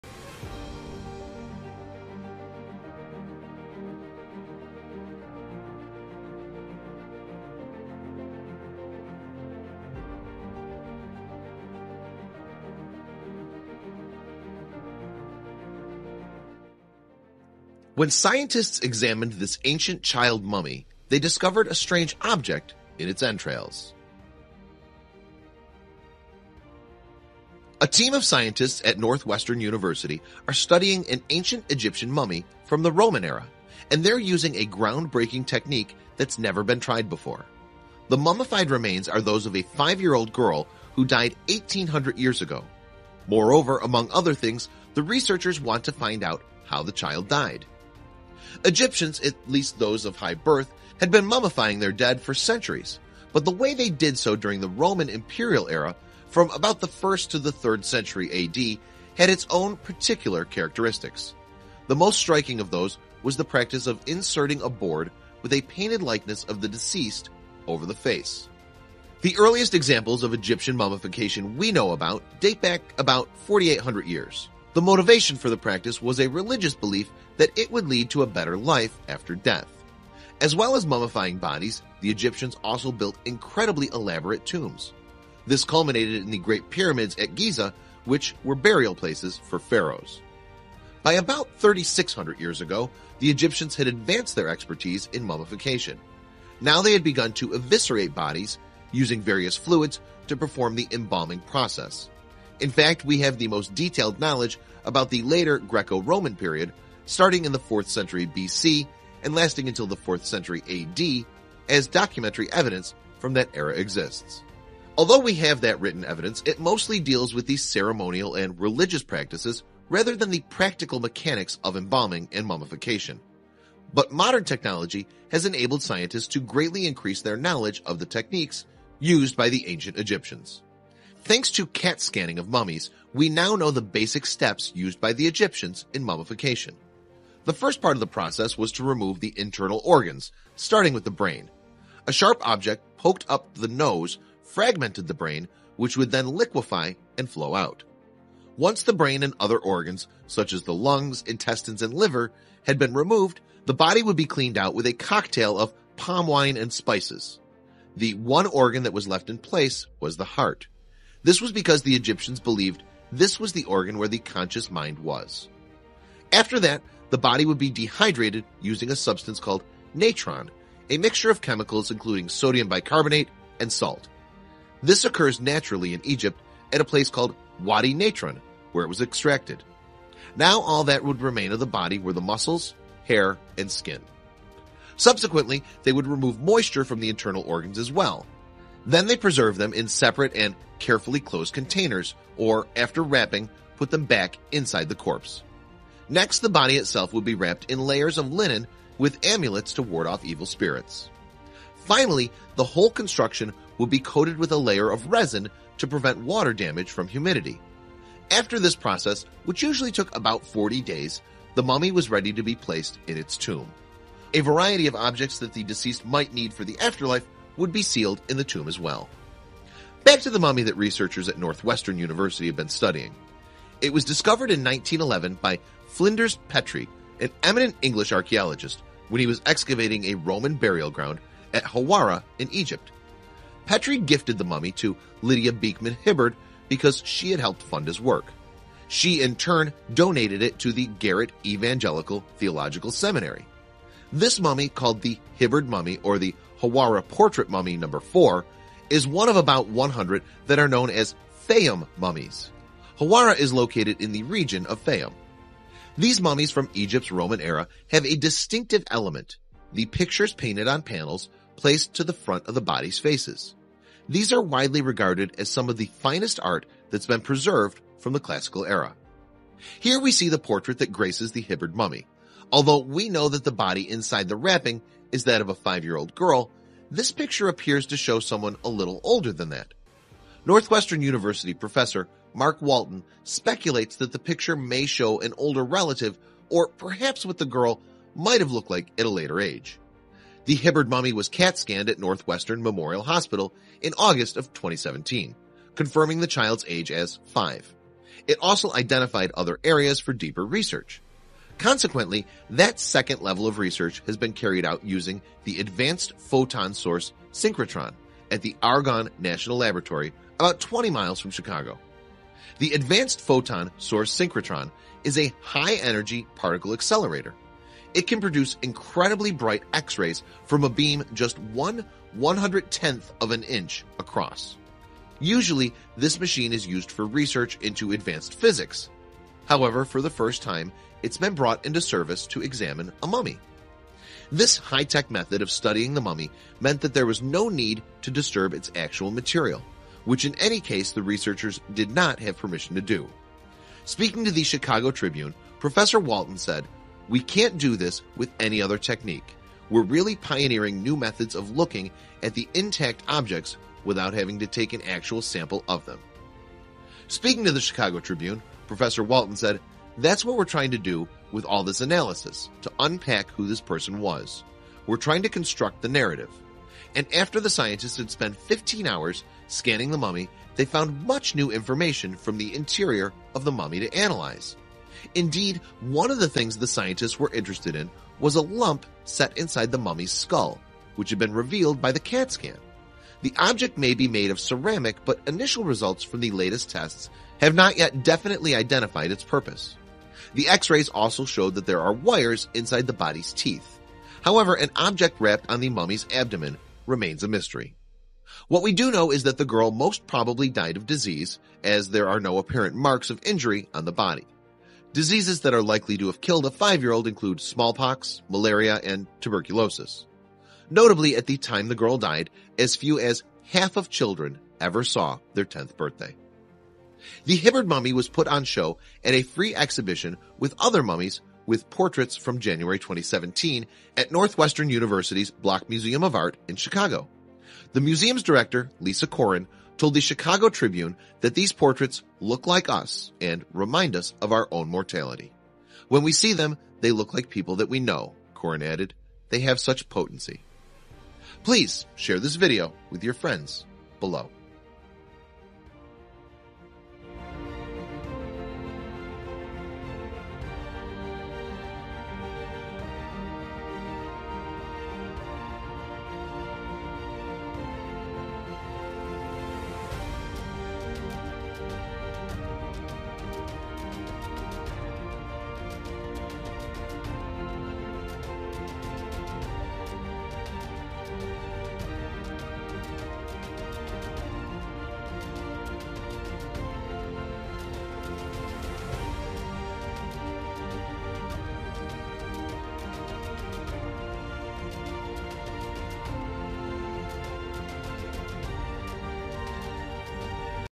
3900-Year-Old Mystery Solved: The Shocking Secrets of Egypt's 'Painted Mummy Girl'
Uncover the fascinating story of "Garret," a 3900-year-old mummy of a 5-year-old Egyptian girl, whose secrets have finally been revealed using cutting-edge technology. Join us as we explore:

• The groundbreaking X-ray scattering technique that unlocked this ancient mystery
• The unique painted portrait on her coffin - one of only 100 known examples
• Her luxurious burial alongside four other mummies, hinting at her high social status
• The intricate wrapping techniques and materials used in her mummification
• Clues about her life, death, and the society she lived in

Discovered in Hawara, Egypt over a century ago, "Garret" remained an enigma until now. Learn how archaeologists from Northwestern University used 3D imaging to peer into the past without disturbing her rest.

We'll also delve into:
- The nearby Hawara pyramid and its connection to Pharaoh Amenemhat III
- The legendary "Labyrinth" palace with its 300 rooms and sacred crocodile mummies
- What this discovery tells us about ancient Egyptian funeral practices and artistry

Don't miss this incredible journey into Egypt's past, where ancient mysteries meet modern science!